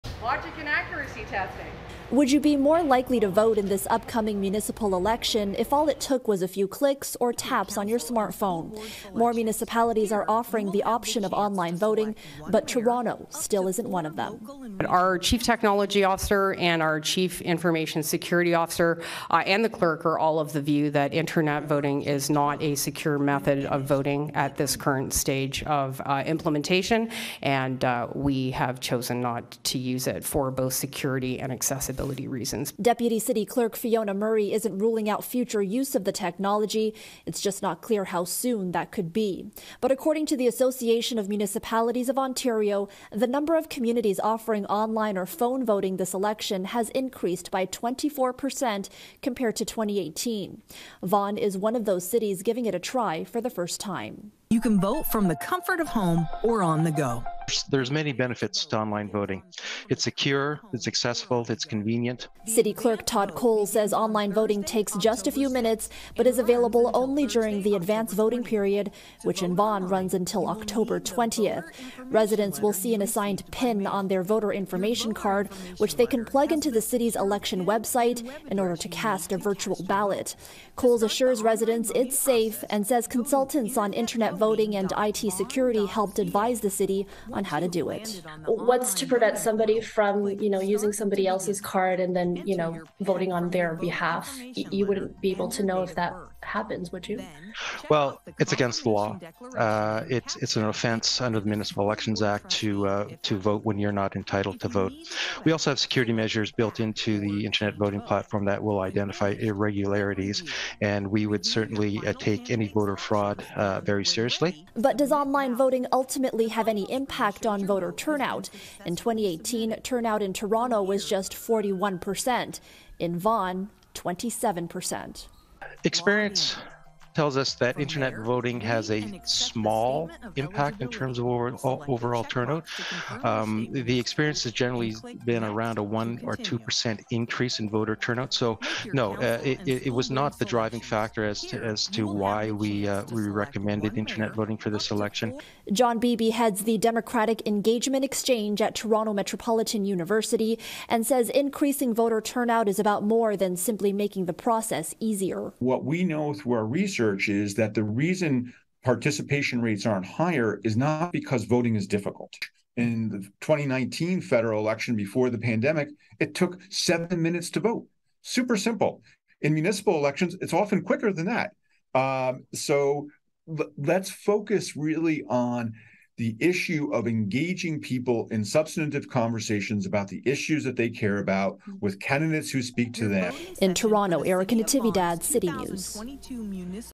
Toronto still not offering online voting in municipal election
Despite more municipalities jumping on board with online voting, Toronto is still holding out. Erica Natividad with what's behind the decision and whether the option could increase voter turnout.